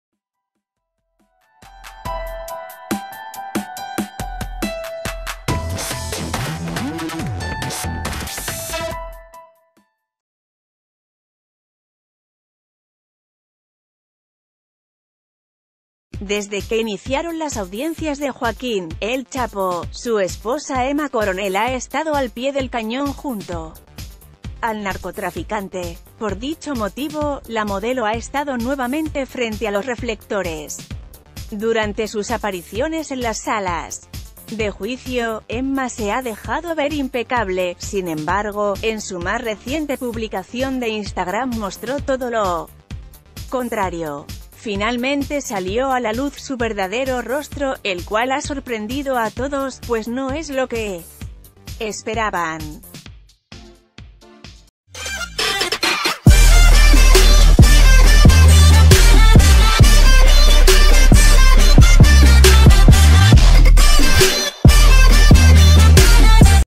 Finalmente sale a la luz el verdadero rostro de Emma Coronel y no es lo que esperaban
Desde que iniciaron las audiencias de Joaquín, ‘El Chapo’, su esposa Emma Coronel ha estado ‘al pie del cañón’ junto al narcotraficante. Por dicho motivo, la modelo ha estado nuevamente frente a los reflectores. Durante sus apariciones en las salas de juicio, Emma se ha dejado ver impecable, sin emb...